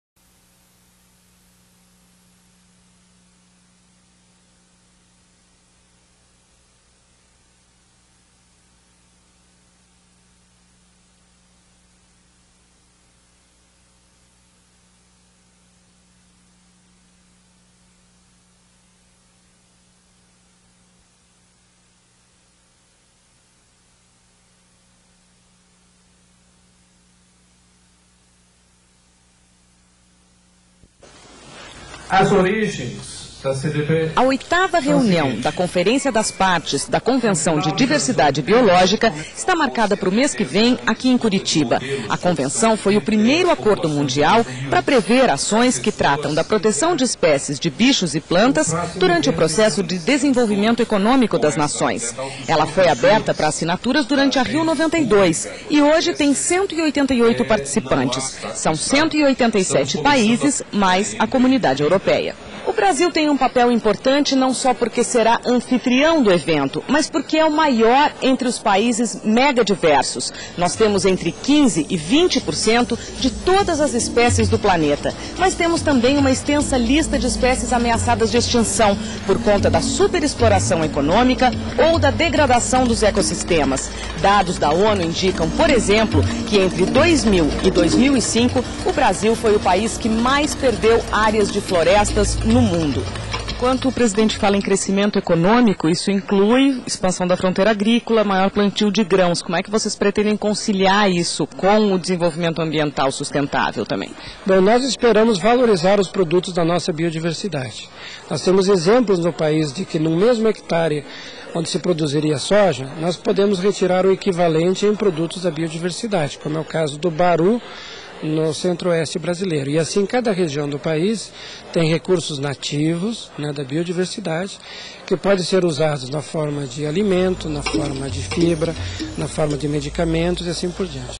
Cerca de 150 jornalistas de todo o Brasil participaram de Seminário sobre dois grandes eventos
A equipe do Repórter Eco participou do Seminário, em Curitiba, promovida pelo Ministério do Meio Ambiente aos jornalistas que vão cobrir em março os dois eventos ambientais mais importantes, depois da Eco-92